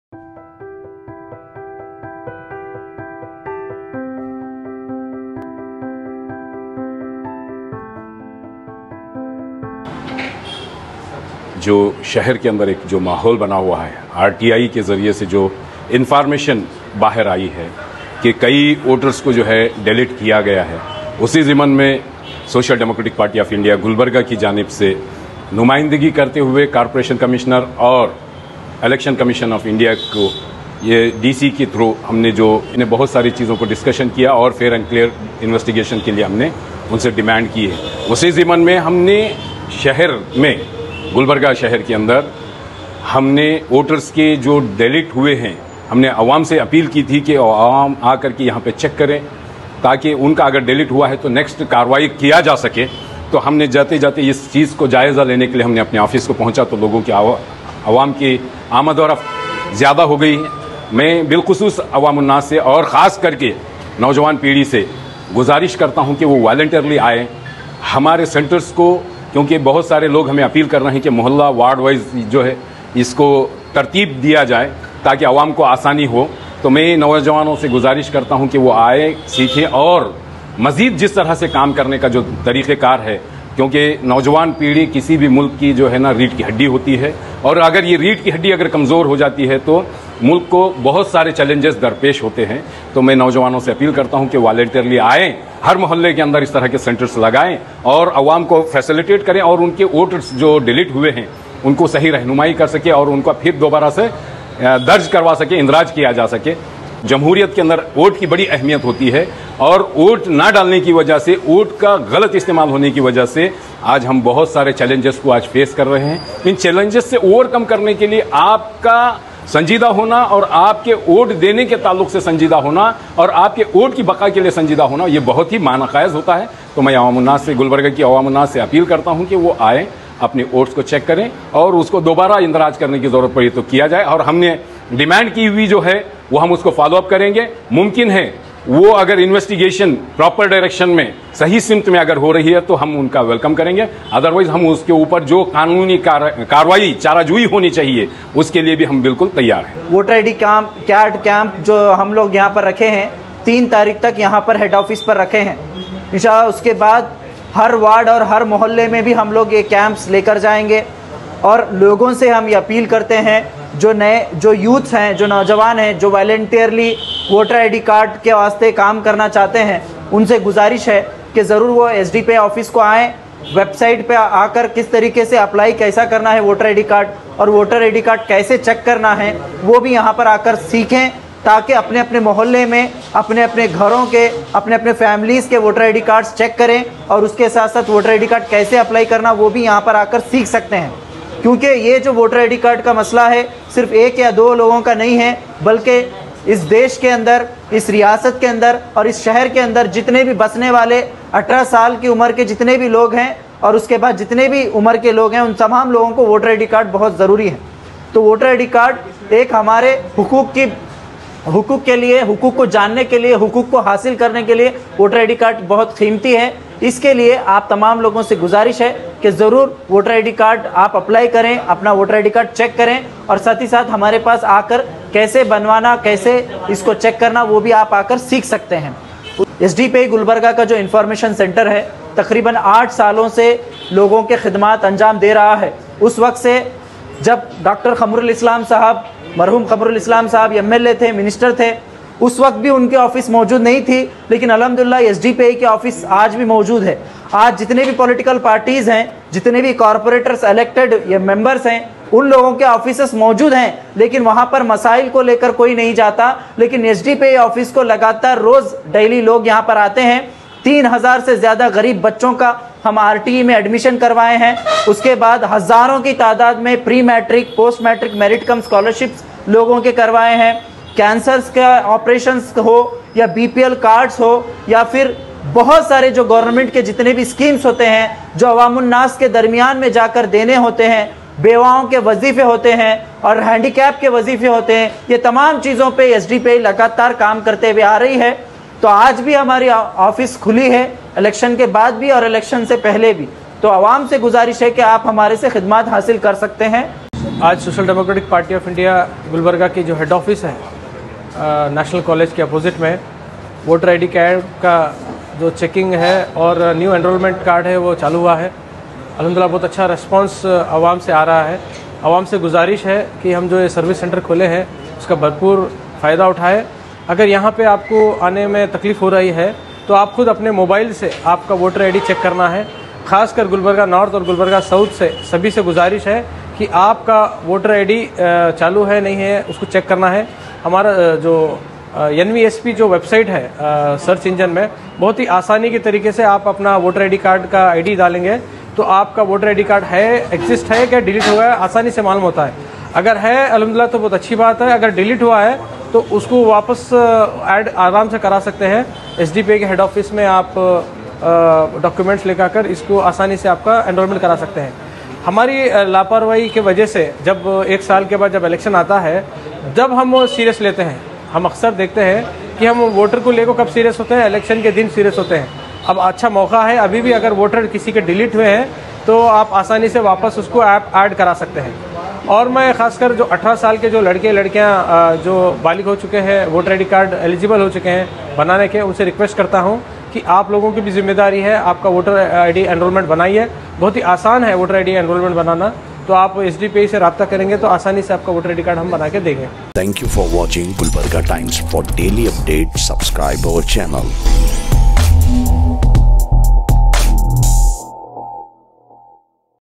VOTER ID ENROLLMENT AND VOTER LIST CHECKING CAMP
VOTER ID ENROLLMENT AND VOTER LIST CHECKING CAMP OGRANISED BY SDPI GULBARGA FROM 28TH JUNE TO 3RD JULY  2022.AT SDPI OFFICE HAFTGUMBAD GULBARGA